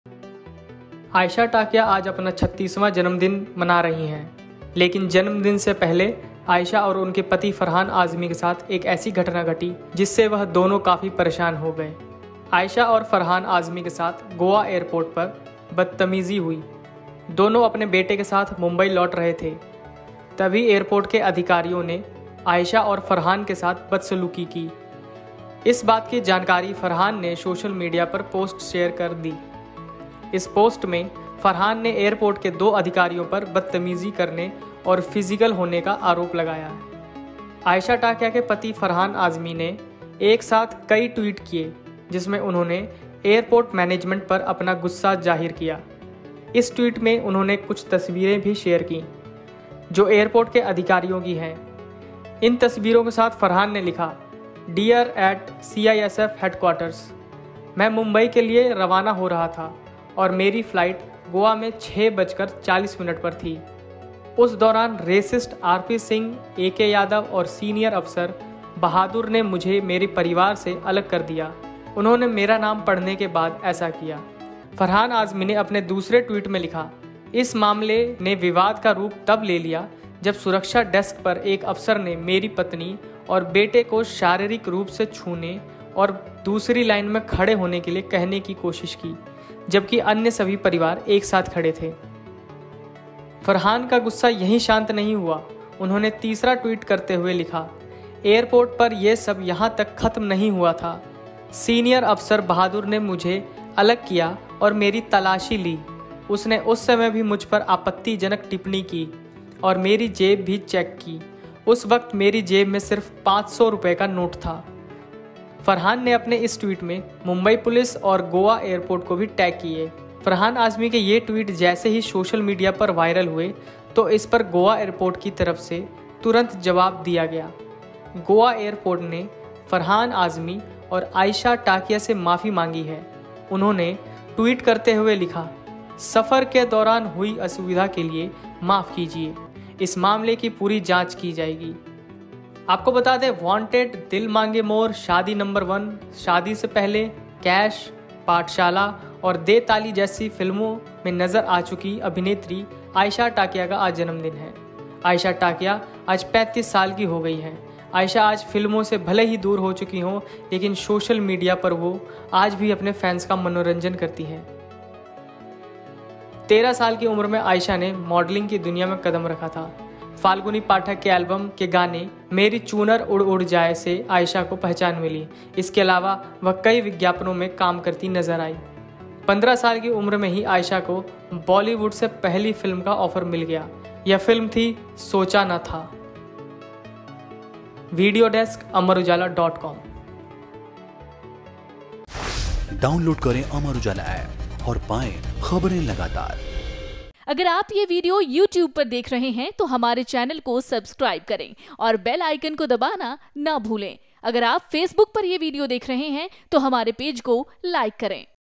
आयशा टाकिया और पति फरहान आजमी के साथ एयरपोर्ट पर हुई बदसलूकी | Ayesha Takia| Farhan Azmi
Ayesha Takia का आज Birthday है। वह अपना 36वां बर्थडे सेलिब्रेट कर रही हैं लेकिन जन्मदिन से पहले आयशा और उनके पति फरहान आजमी के साथ एक ऐसी घटना घटी, जिससे वह दोनों काफी परेशान हो गए। आयशा और फरहान आजमी के साथ गोवा एयरपोर्ट पर बदतमीजी हुई। दोनों अपने बेटे के साथ मुंबई लौट रहे थे, तभी एयरपोर्ट के अधिकारियों ने आयशा और फरहान के साथ बदसलूकी की। इस बात की जानकारी फरहान ने सोशल मीडिया पर पोस्ट शेयर कर दी। इस पोस्ट में फरहान ने एयरपोर्ट के दो अधिकारियों पर बदतमीजी करने और फिजिकल होने का आरोप लगाया है।